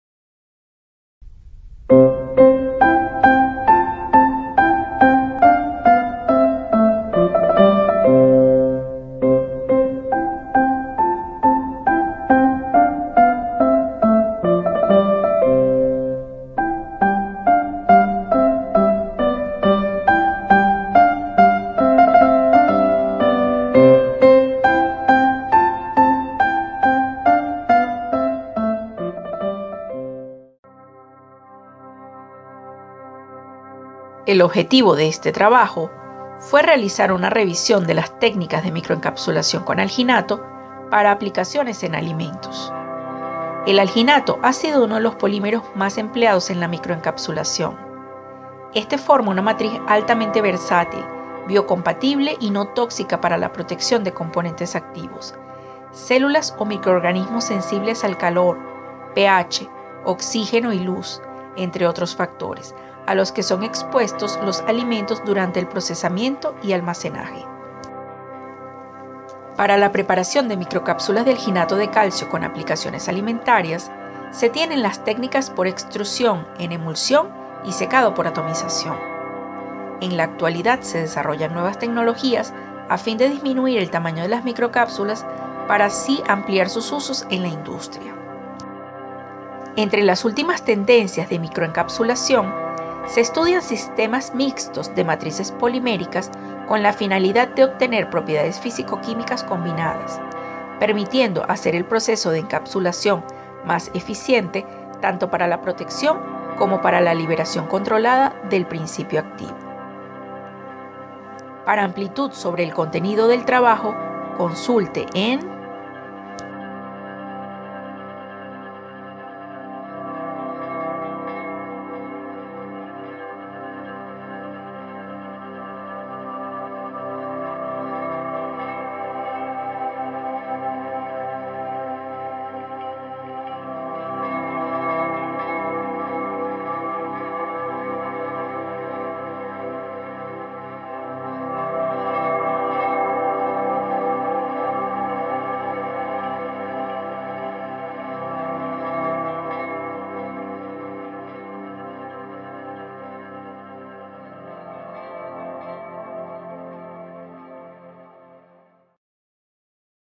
Microencapsulación con alginato en alimentos (RVCTA)
Microencapsulación con alginato en alimentos. Técnicas y aplicaciones. AUTORAS: Bryshila Lupo Pasin, Carmen González Azón, Alicia Maestro Garriga. VENEZUELA-ESPAÑA. (En Volumen 3 - Número 1).